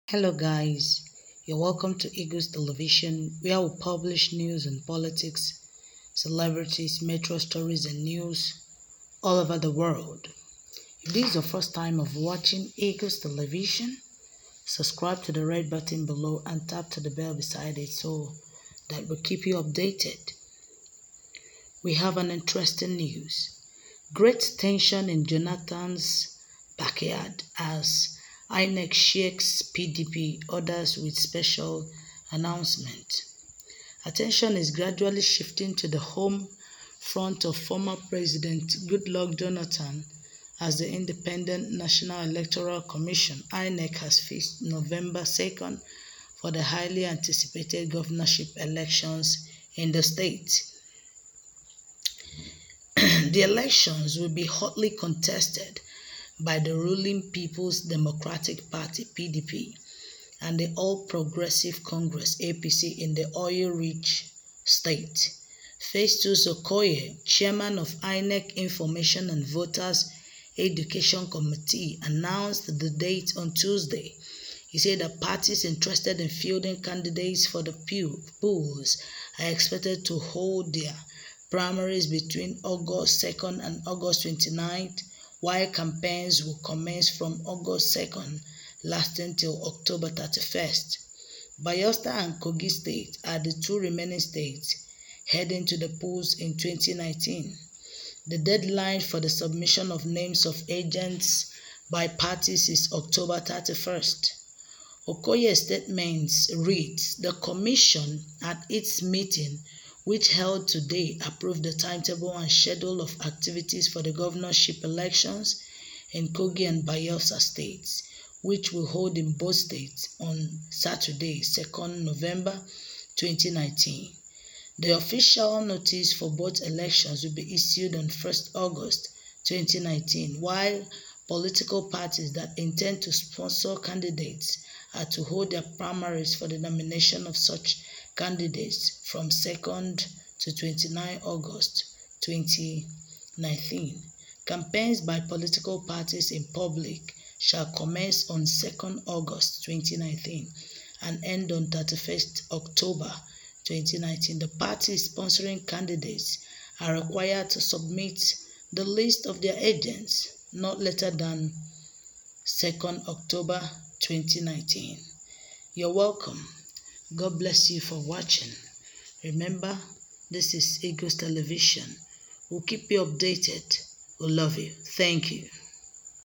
Great tension in Jonathan's backyard,
Great tension in Jonathan's backyard,as INEC shakes PDP,others with special announcement.